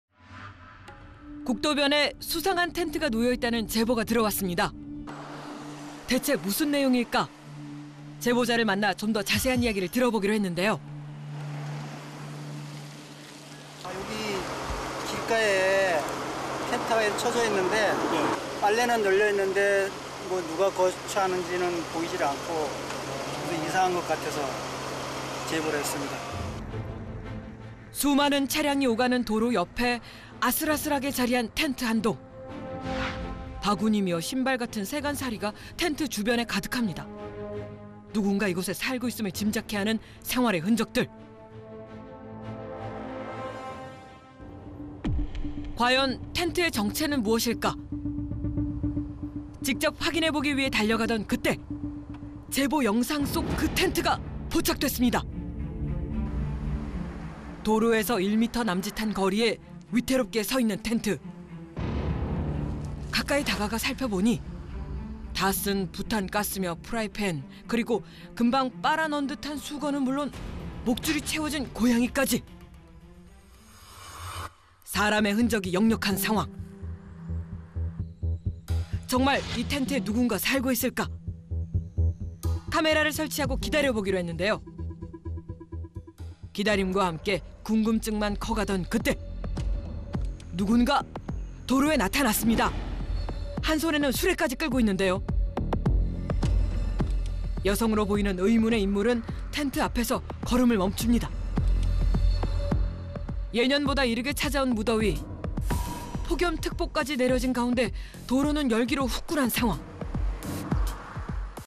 국도변에 수상한 텐트가 있다?!
길가 텐트에 정말 사람이 살고 있을까?